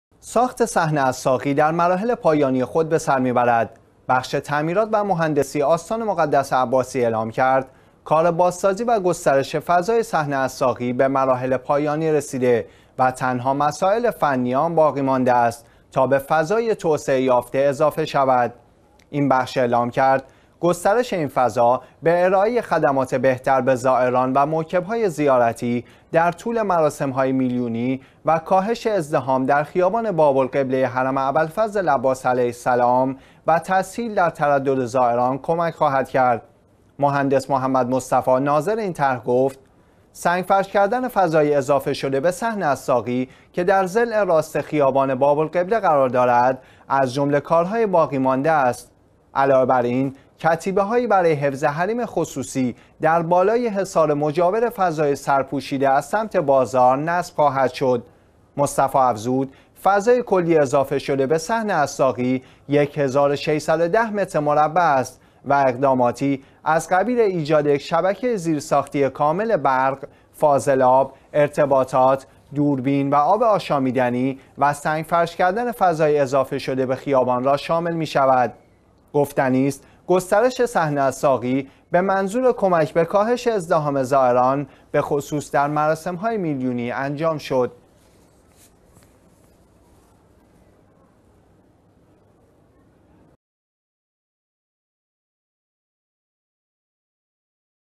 گسترش فضای صحن الساقی حرم در مراحل پایانی
ساخت صحن الساقی در مراحل پایانی خود به سر می‌برد.
بخش تعمیرات و مهندسی  آستان مقدس عباسی اعلام کرد: کار بازسازی و گسترش فضای صحن الساقی به مراحل پایانی رسیده و تنها مسایل فنی آن باقی مانده است تا به فضای توسعه یافته اضافه شود.
این بخش اعلام کرد: گسترش این فضا به ارائه خدمات بهتر به زائران و موکب های زیارتی در طول مراسم های میلیونی و کاهش ازدحام در خیابان باب القبله حرم ابوالفضل العباس علیه السلام و تسهیل در ترد زائران کمک خواهد کرد.
مهندس محمد مصطفی، ناظر این طرح گفت: سنگفرش کردن فضای اضافه شده به صحن الساقی که در ضلع راست خیابان باب القبله قرار دارد، از جمله کارهای باقی مانده است.
علاوه براین، کتیبه هایی برای حفظ حریم خصوصی در بالای حصار مجاور فضای سرپوشیده از سمت بازارها نصب خواهد شد.
مصطفی افزود: فضای کلی اضافه شده به صحن الساقی یک هزار و 610 متر مربع است و اقداماتی از قبیل ایجاد یک شبکه زیرساختی کامل (برق، فاضلاب، ارتباطات، دوربین و آب شامیدنی) و سنگ فرش کردن فضای اضافه شده به خیابان را شامل می شود.
گفتنی است، گسترش صحن الساقی به منظور کمک به کاهش ازدحام زائران به خصوص در مراسم های میلیونی انجام شد.